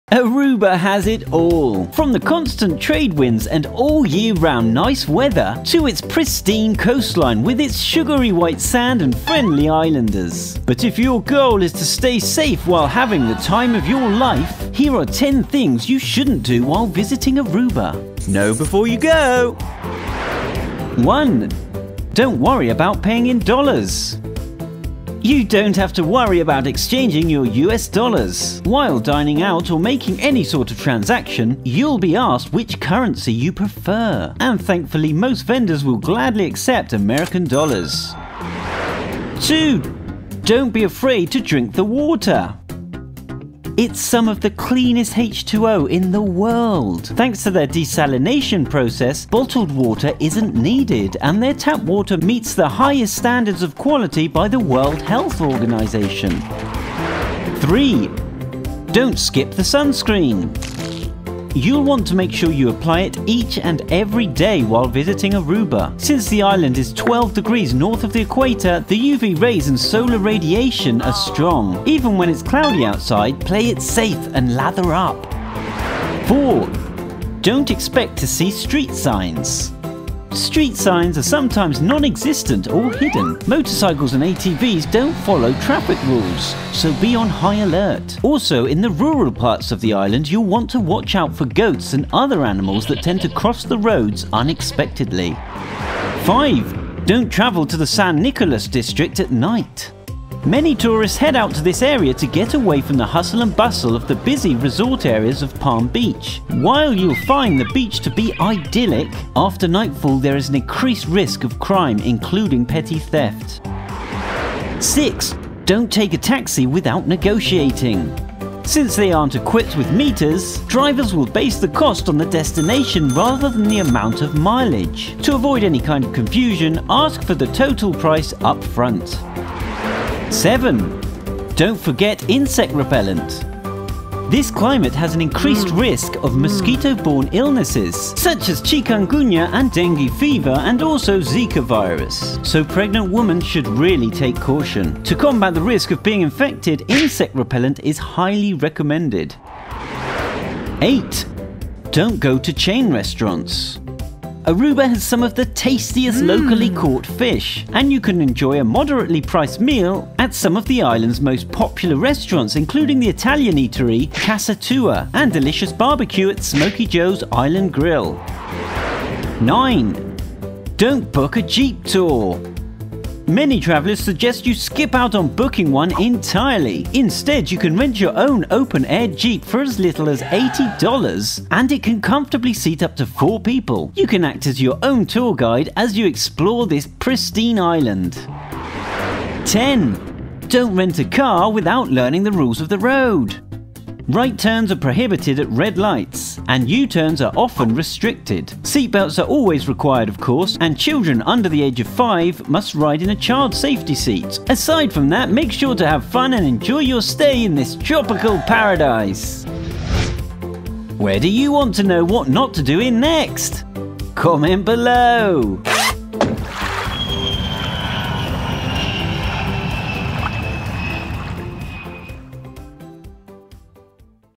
10 Things NOT to Do in Aruba
Aruba has it all – from the constant trade winds and year-round nice weather to its pristine coastline with its sugary white sand and friendly islanders. But if your goal is to stay safe while having the time of your life, here are 10 things you shouldn’t do while visiting Aruba.

1. Don’t Worry About Paying in USD
You don’t have to worry about exchanging your USD (Visual: Exchange Office). While dining out or making any sort of transaction, you’ll be asked which currency you prefer. And, thankfully, most vendors will gladly accept American dollars.

2. Don’t Be Afraid to Drink the Water
It’s some of the cleanest H2O in the world. Thanks to their desalination process, bottled water isn’t needed, and their tap water meets the highest standards of quality by the World Health Organization..

3. Don’t Skip the Sunscreen
You’ll want to make sure you apply it each and every day while visiting Aruba. Since the island is 12 degrees north of the equator, the UV rays and solar radiation is strong. Even when it’s cloudy outside, play it safe and lather up!

4. Don’t Expect to See Street Signs
Street signs are sometimes nonexistent or hidden. Motorcycles and ATVs don’t follow traffic rules, so be on high alert. Also, in the rural parts of the island, you’ll want to watch out for goats and other animals that tend to cross the roads unexpectedly.

5. Don’t Travel to the San Nicolas District at Night
Many tourists head out to this area to get away from the hustle and bustle of the busy resort areas of Palm Beach. While you’ll find the beach to be idyllic, after nightfall there is an increased risk of crime, including petty theft.

6. Don’t Take a Taxi Without Negotiating
Since they aren’t equipped with meters (Visual: Taximeter), drivers will base the cost on the destination rather than the amount of mileage. To avoid any kind of confusion, ask for the total price upfront.

7. Don’t Forget Insect Repellent
This climate has an increased risk of mosquito-borne illnesses, such as chikungunya and dengue fever,  and also Zika virus, so pregnant women should really take caution. To combat the risk of being infected, insect repellent is highly recommended.

8. Don’t Go to Chain Restaurants (Visual: Hard Rock Cafe)

Aruba has some of the tastiest locally caught fish, and you can enjoy a moderately priced meal at some of the island’s most popular restaurants, including the Italian eatery Casa Tua and delicious BBQ at Smokey Joe’s Island Grill.

9. Don’t Book a Jeep Tour

Many travelers suggest you skip out on booking one entirely. Instead, you can rent your own open-air jeep for as little as $80, and it can comfortably seat up to 4 people. You can act as your own tour guide as you explore the pristine island.

10. Don’t Rent a Car Without Learning the Rules of the Road
Right turns are prohibited at red lights, and U-turns are often restricted. Seat belts are always required, of course, and children under the age of 5 must ride in a child safety seat. Aside from that, make sure to have fun and enjoy your stay at this tropical paradise!

Where do you want to know what not to do in next?